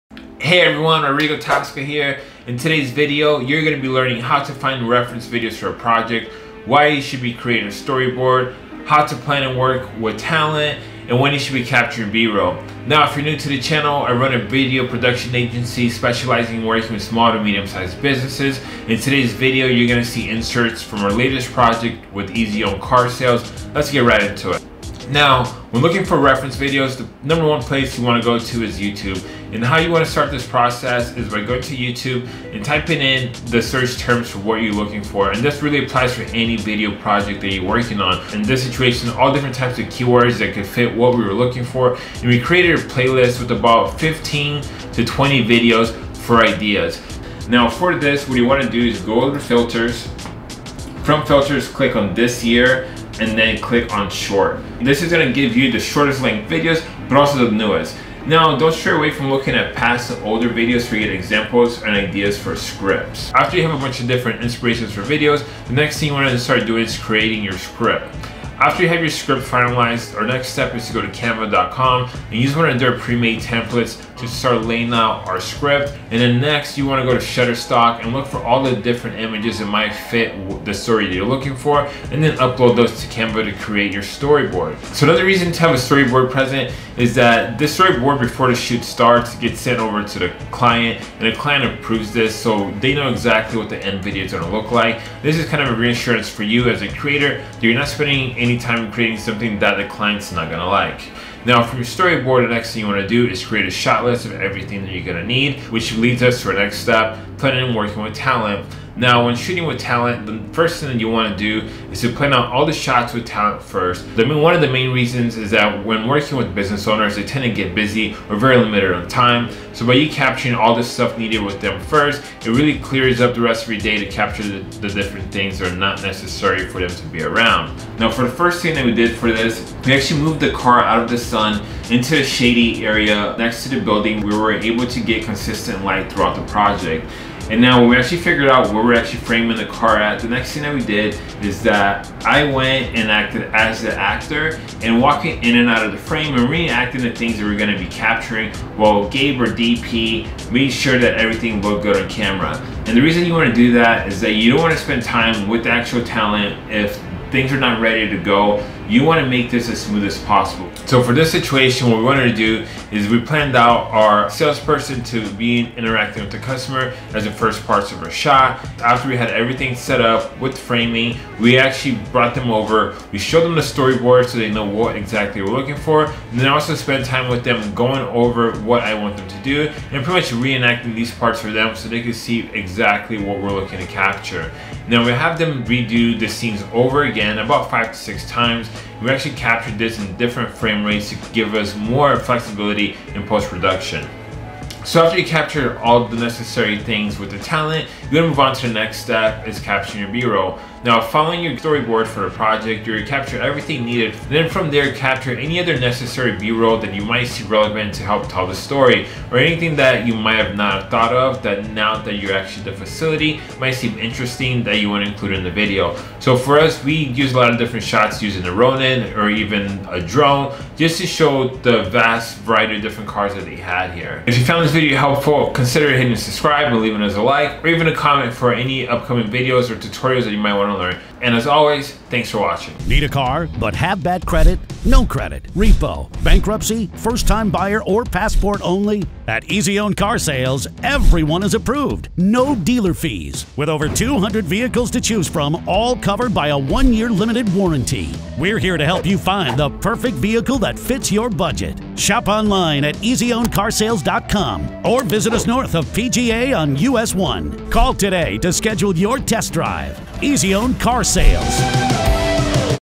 How to storyboard and shoot a commercial (2019)
How to storyboard and shoot a commercial

How to find inspiration on Youtube for video projects. Create a playlist with all the inspo videos. Then use Canva to create a storyboard by utilizing free images from Shutterstock for a mood board.

0:00 - Introduction
0:24 - Find reference videos
1:03 - Script creation
1:09 - Storyboard creation
1:16 - Select your images
1:41 - Create your shot-list
1:48 - Cast and schedule the talent
2:08 - Get consistent lighting
2:20 - Rehearsal
3:15 - Capture b-roll footage